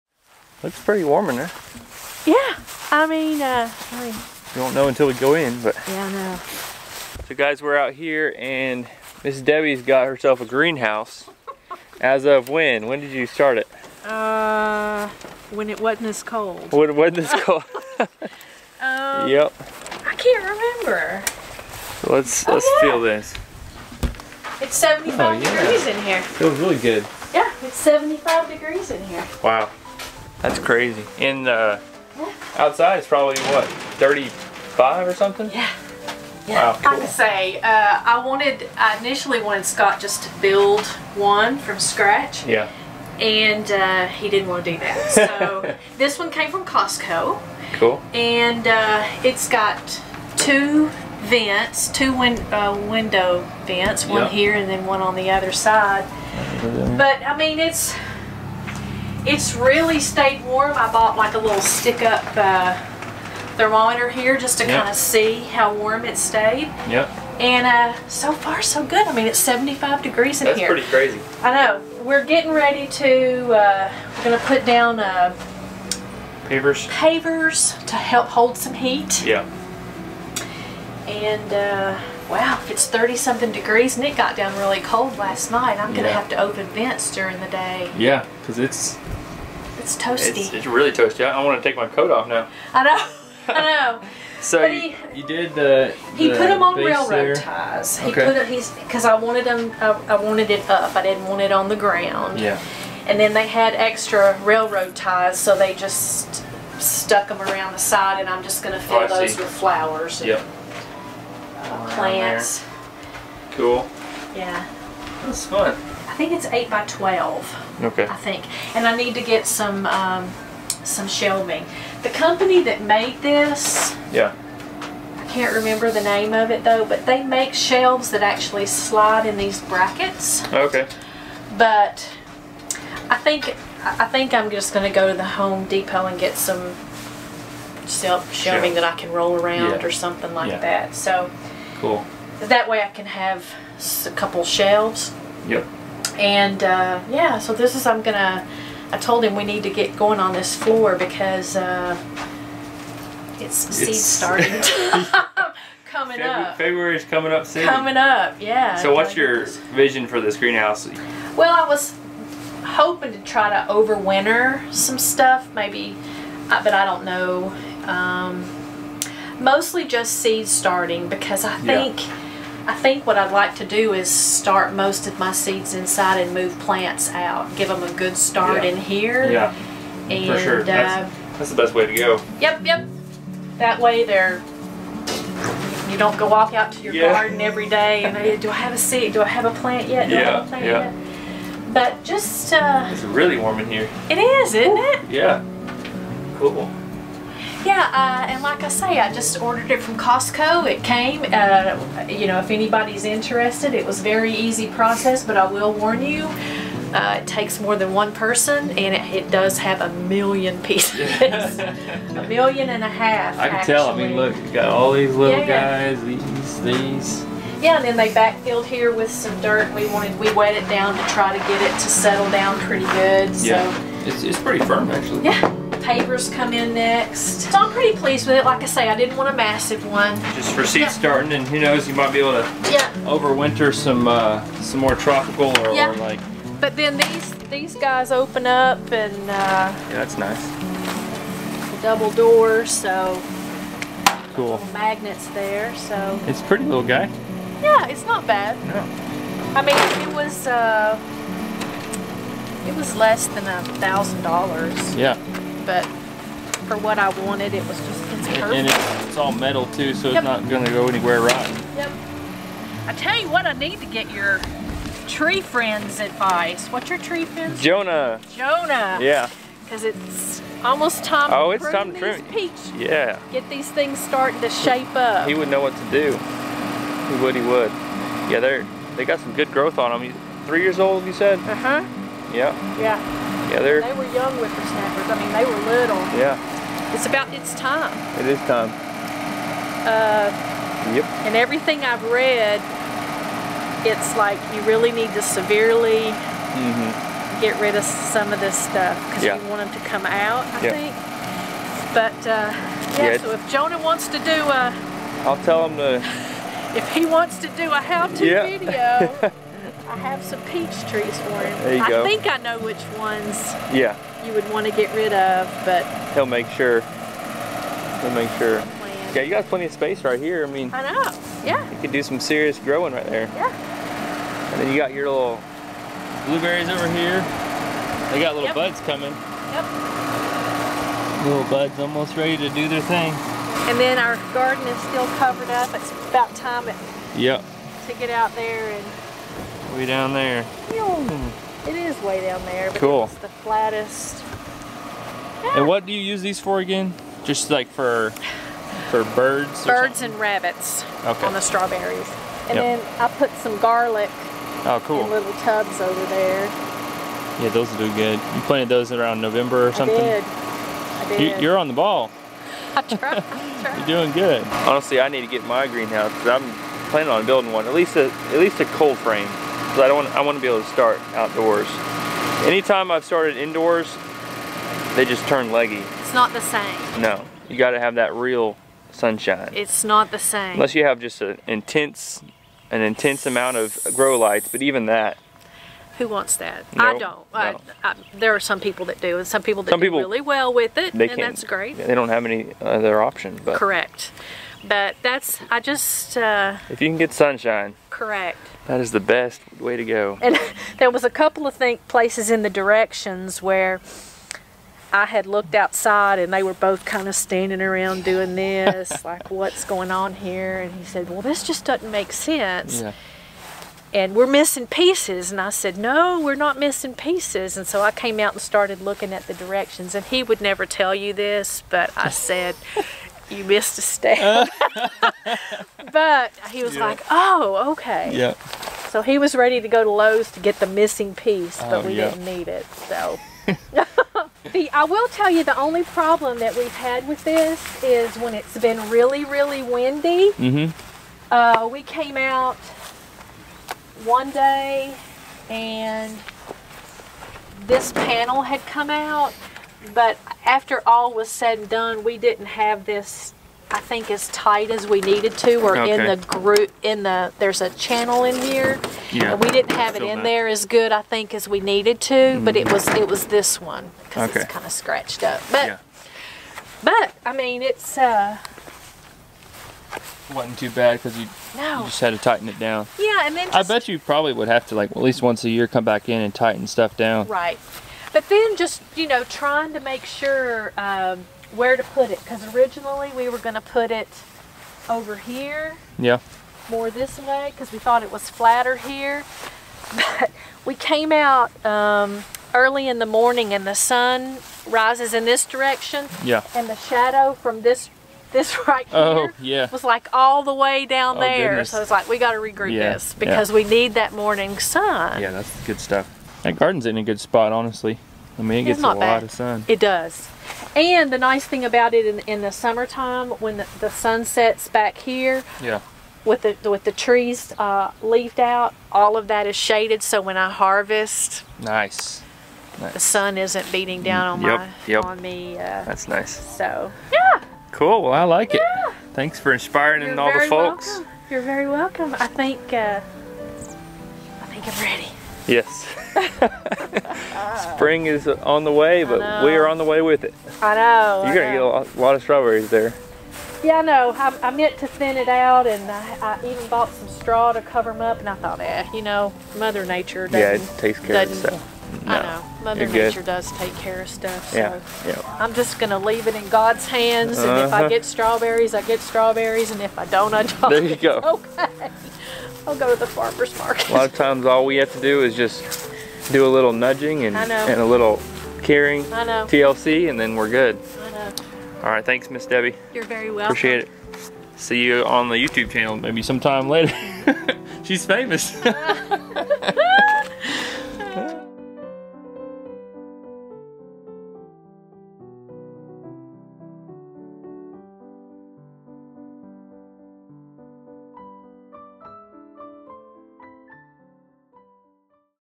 Brand NEW GREENHOUSE Tour | Her Story of the Install
Hey folks! In this video Mrs. Debbie gives us a tour of he new greenhouse along with a few tips and tricks she is utilizing to optimize warmth in the winter.
It's always fun seeing what Mrs. Debbie is growing around the house!
They're aaaaaalmost done and we will be completing them in the next video.
Blessings
PS.

- Benj aka Gardenguy